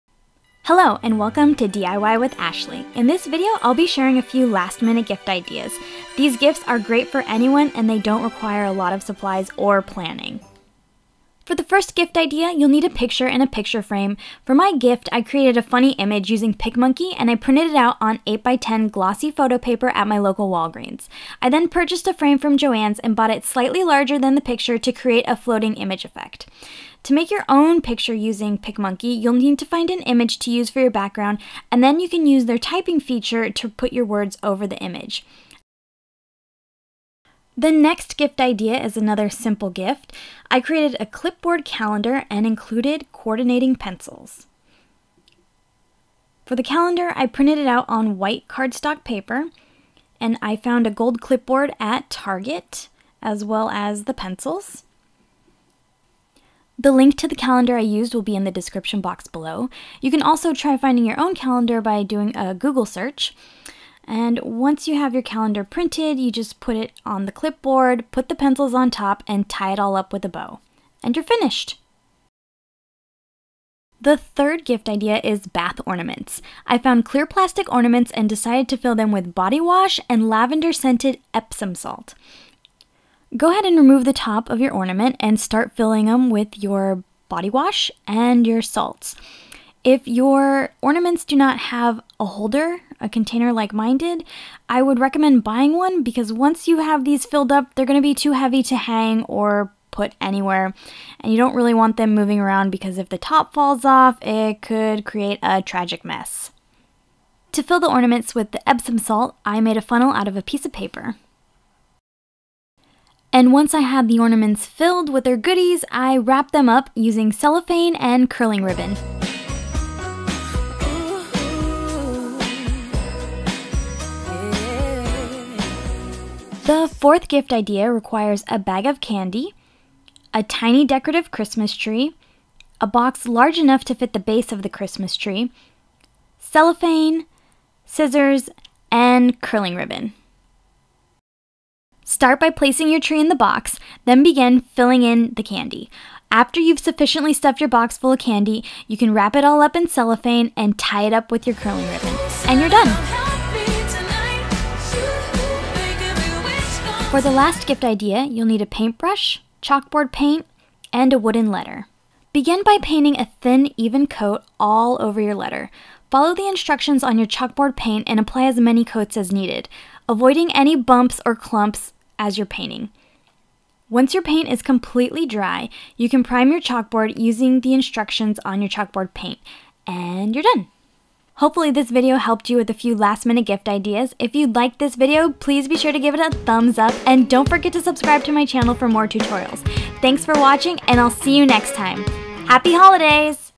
DIY Last Minute Holiday Gift Ideas
In this video I'll show you 5 easy last minute gift ideas for anyone on your holiday list!

For the clipboard calendar, you can find the printable 2017 calendar

I hope this video helps you with your last minute holiday gifts.

Happy Holidays!